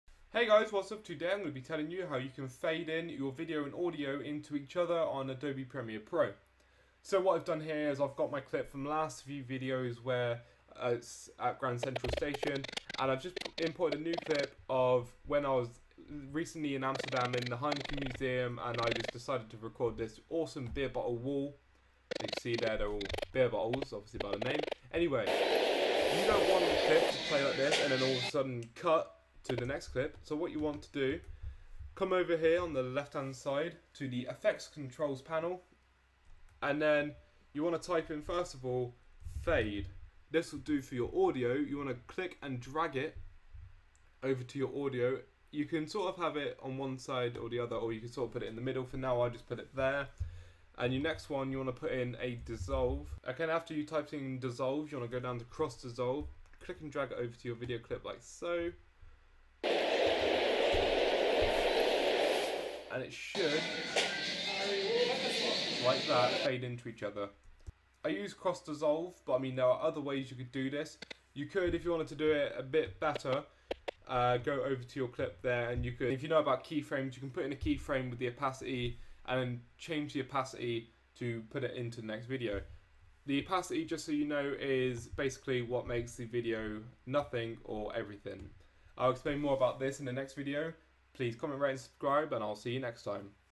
How To Fade Audio/Video Into Each Other In Adobe Premiere Pro
This video will show you how I personally fade audio and video, there are many ways, this is one of them
Thanks!